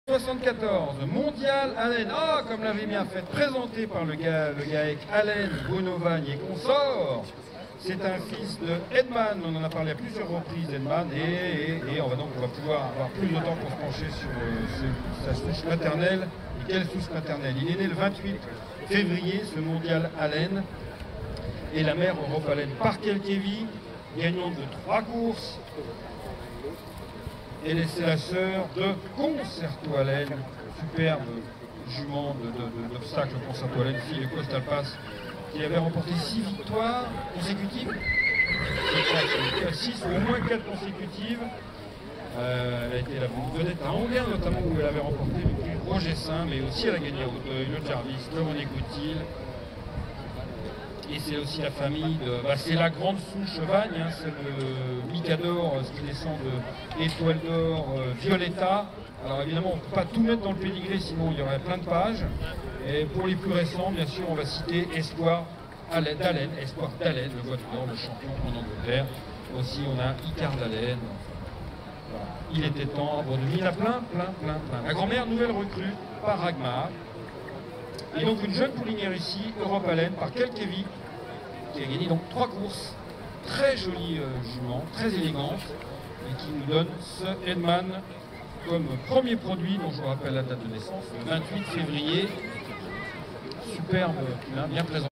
AQPS Foal Show 2022 - Lot 74 - MONDIAL ALLEN
Organisé par GalopAllier avec le précieux concours de l'Union du Centre-Est, l'AQPS Foal Show de Moulins s'enrichit déjà pour sa 2e édition d'un présentation "breeze up" de pouliches de 2 ans. Au total, 80 lots seront proposés aux socio-professionnnels français et étrangers.